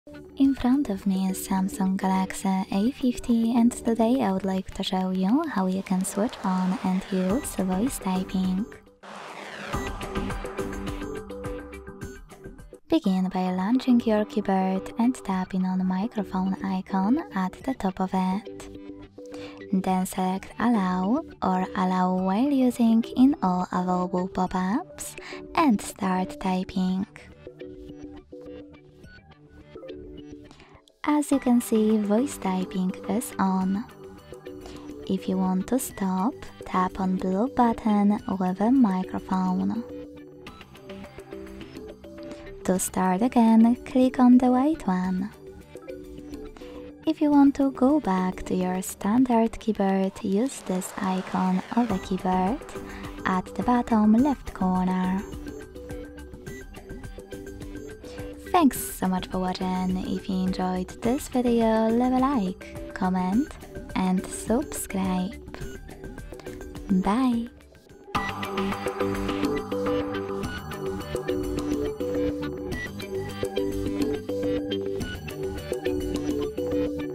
Samsung Galaxy A50 - How to Turn ON and Use Voice Typing? Create / Write Messages via Voice!
Hello Guys! Explore the convenience of voice typing on your Samsung Galaxy A50. By turning on this feature, you can compose messages and other text inputs simply by speaking, which can save you time and make typing easier, especially when on the go.

How to Turn ON Voice Typing in Samsung Galaxy A50?
How to Enable Voice Typing in Samsung Galaxy A50 Smartphone?
How to Activate Voice Typing in Samsung Galaxy A50 Phone?